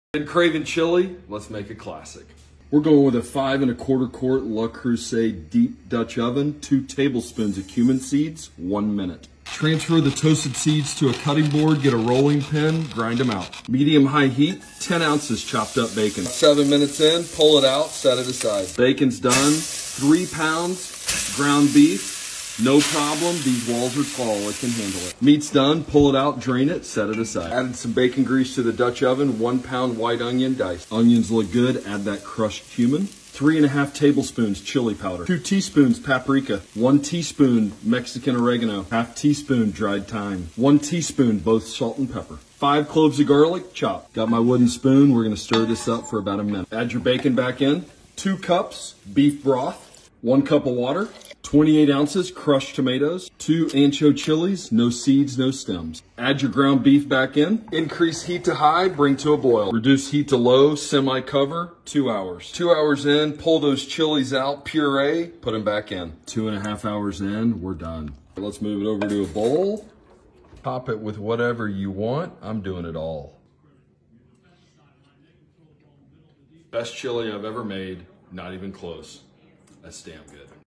Classic Chili con Carne in the Deep Oven
Deep Oven recipe inspiration: Classic Chili con Carne, courtesy of 3 Pieces of Pecan.

Ingredients:
2 Tbsp whole cumin seeds
10oz bacon, chopped
3 lbs ground beef, beef chuck, buffalo or venison
2 white onions, diced
3.5 Tbsp chili powder
2 tsp paprika
1 tsp Mexican oregano
1 tsp black pepper
1/2 tsp dried thyme
1/2 tsp salt
6 cloves garlic, minced
2 cups beef broth
28 oz crushed tomatoes
2 ancho chilis, stems and seeds removed
Water as needed to reach your desired consistency

Instructions:
1) Heat the Deep Oven over medium heat. Toast cumin seeds for 1 minute or less until fragrant, then move to them to a cutting board and crush with a rolling pin.
2) Increase heat to medium-high. Fry bacon and remove.
3) Cook beef over medium-high until evenly browned, then remove.
4) Reduce heat to medium. Add onions in remaining drippings and saute until slightly brown.
5) Add tomatoes and all seasonings plus garlic. Stir for 1-2 minutes. Add bacon, broth, tomatoes, a splash of water, ancho chilis and beef. Bring to a boil, then reduce to a simmer. Partly cover and cook for 2 hours, adding a bit of water as needed.
6) Remove ancho chilis, purée them in a blender, and return them to the Deep Oven.
7) Simmer 30 minutes to an hour, then it's ready to serve.
8) The leftovers are great as well, just add water as needed to adjust the consistency!

Recipe inspired by Rob Walsh's Classic Chili con Carne, via New York Times Cooking.